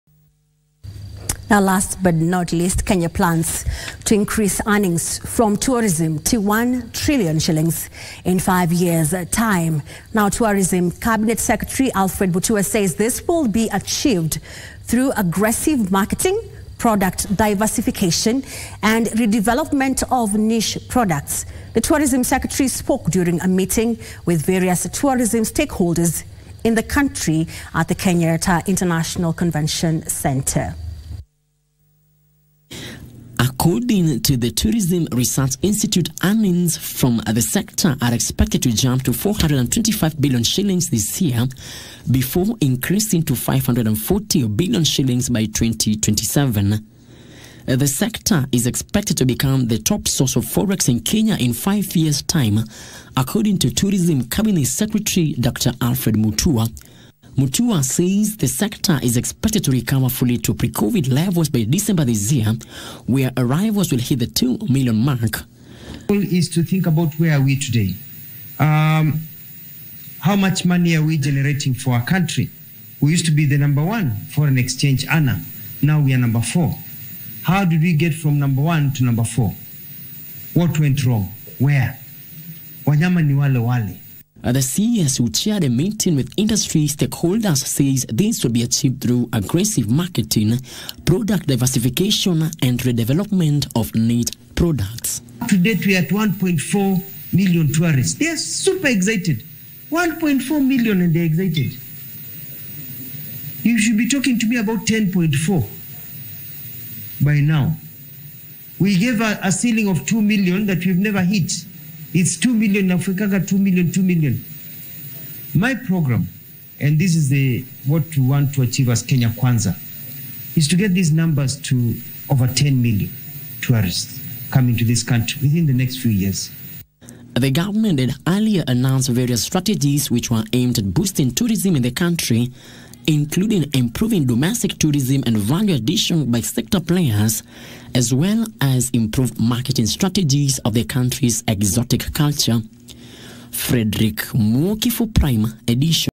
Kenya plans to increase earnings from tourism to 1 Trillion shillings in 5 years
Kenya is embarking on an ambitious plan to significantly boost its tourism industry with the goal of increasing earnings to 1 trillion shillings within the next five years. This strategic move aims to capitalize on the country's abundant natural beauty, wildlife, cultural heritage, and diverse landscapes to attract a greater number of tourists and generate substantial revenue.